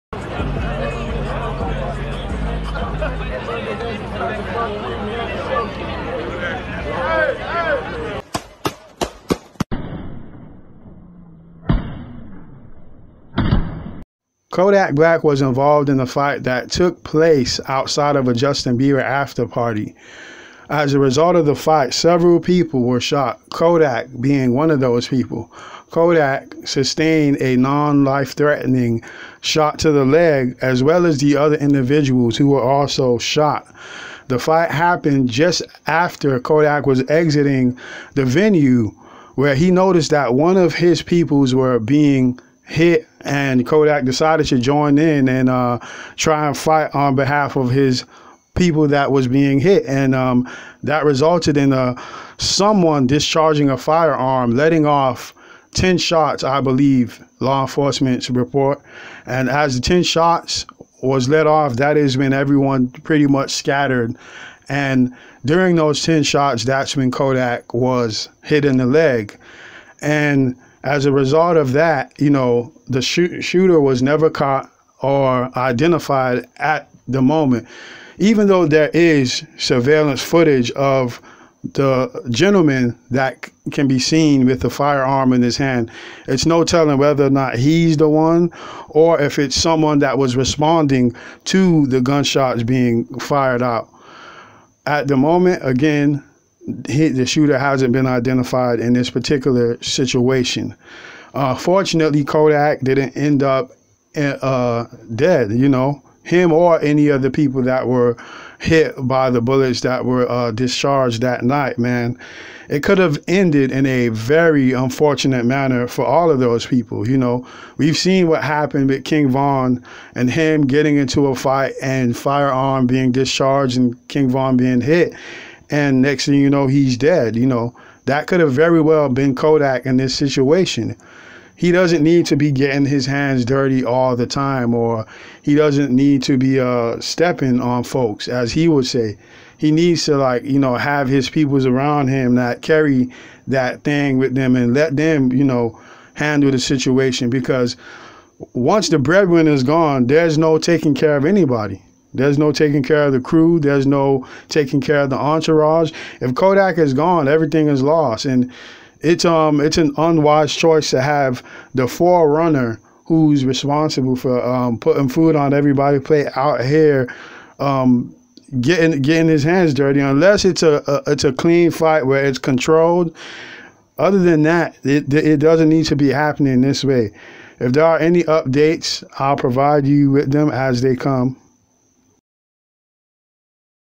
Kodak Black Shot In The Leg After Fight Outside Of Justin Bieber After-party
Kodak Black Hit In The Leg After Fight Outside Of Justin Bieber After-party